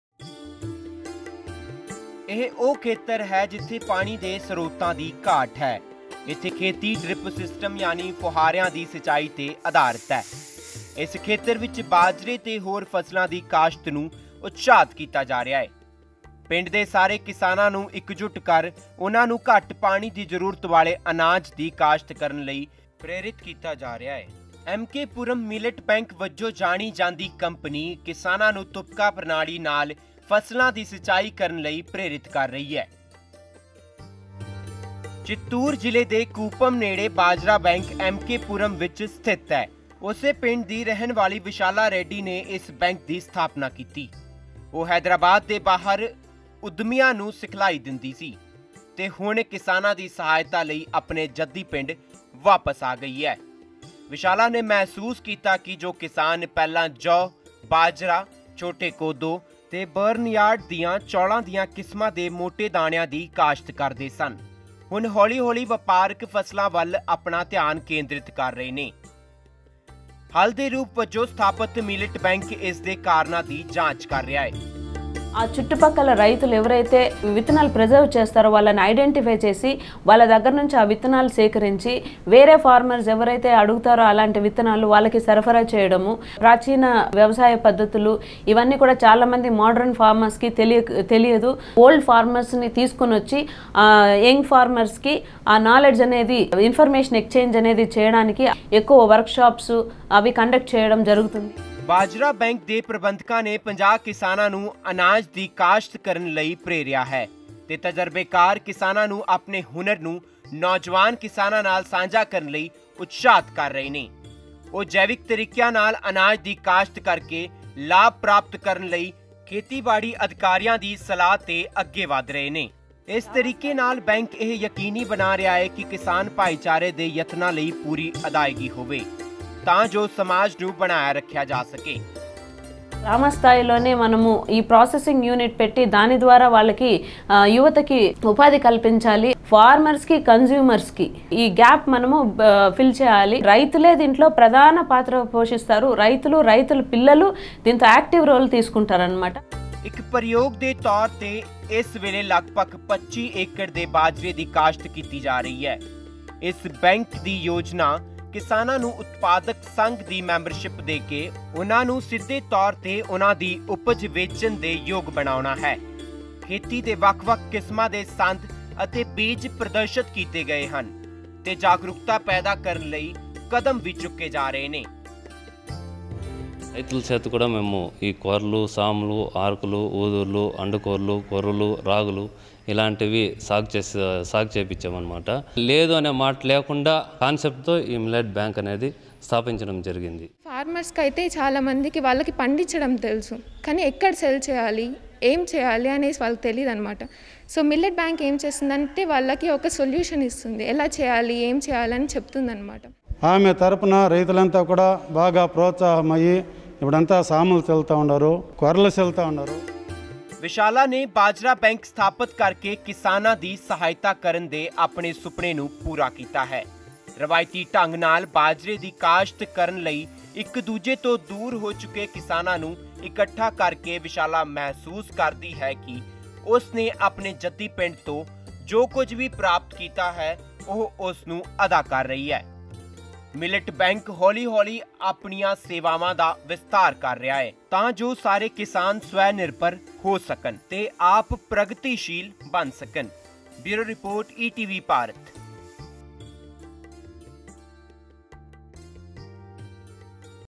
ਰਾਵਾਇਤੀ ਖੇਤੀ ਨੂੰ ਬਚਾਉਣ ਲਈ ਸਥਾਪਤ ਕੀਤਾ "ਬਾਜਰਾ ਬੈਂਕ" | Vishala sets up Millet Bank | ETV Bharat Punjab
ਪਾਣੀ ਦੇ ਸਰੋਤਾਂ ਦੀ ਘਾਟ ਕਾਰਨ ਖ਼ਤਮ ਹੋ ਰਹੀ ਰਵਾਇਤੀ ਖੇਤੀ ਦੀ ਸਾਂਭ ਲਈ ਆਂਧਰਾ ਪ੍ਰਦੇਸ਼ ਦੇ ਚਿਤੂਰ ਵਿਸ਼ਾਲਾ ਰੈਡੀ ਅੱਗੇ ਆਈ, ਜਿਨ੍ਹਾਂ ਵੱਲੋਂ ਕਿਸਾਨਾਂ ਲਈ ਬਾਜਰਾ ਬੈਂਕ ਦੀ ਸਥਾਪਨਾ ਕੀਤਾ ਗਈ। ਇਹ ਸਾਰੀ ਖੇਤੀ ਡ੍ਰਿਪ ਸਿਸਟਮ ਯਾਨੀ ਫੁਹਾਰਿਆਂ ਦੀ ਸਿੰਜਾਈ 'ਤੇ ਹੀ ਅਧਾਰਿਤ ਹੈ। ਵਿਸ਼ਾਲਾ ਦੇ ਇਸੇ ਉਪਰਾਲੇ ਰਾਹੀਂ ਬਾਜਰੇ ਤੇ ਹੋਰ ਫਸਲਾਂ ਦੀ ਕਾਸ਼ਤ ਨੂੰ ਉਤਸ਼ਾਹਤ ਕੀਤਾ ਜਾ ਰਿਹਾ ਹੈ।